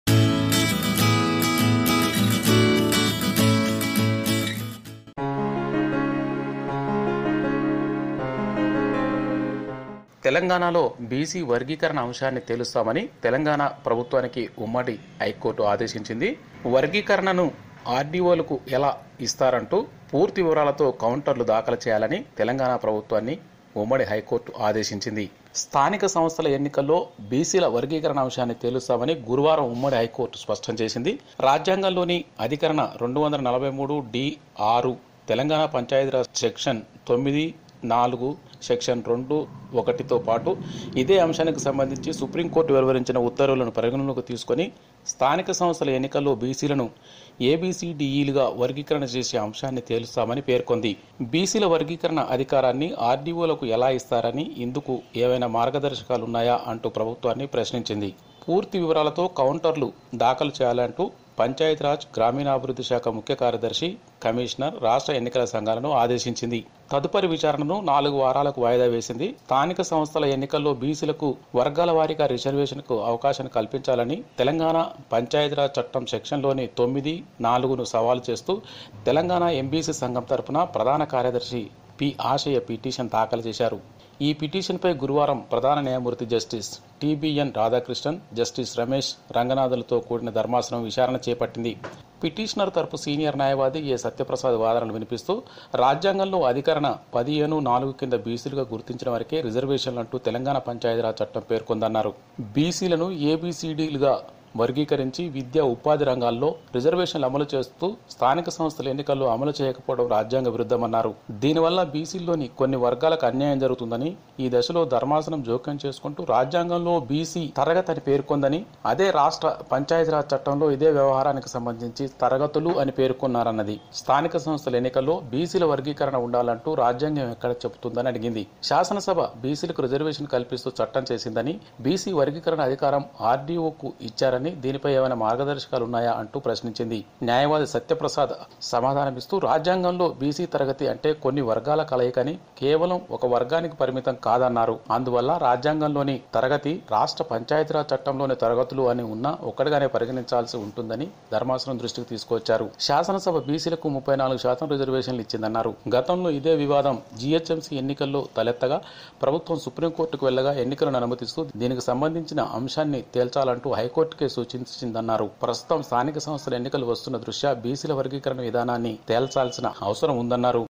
తెలంగాణ ప్రభుత్వానికి హైకోర్టు ఆదేశం | BC Resevations | High Court | Telanga State | MBC Times
తెలంగాణ ప్రభుత్వానికి హైకోర్టు ఆదేశం | BC Resevations | High Court | Telanga State | MBC Times. hight court stay on bc reservations. telangana grama panchayath elections 2018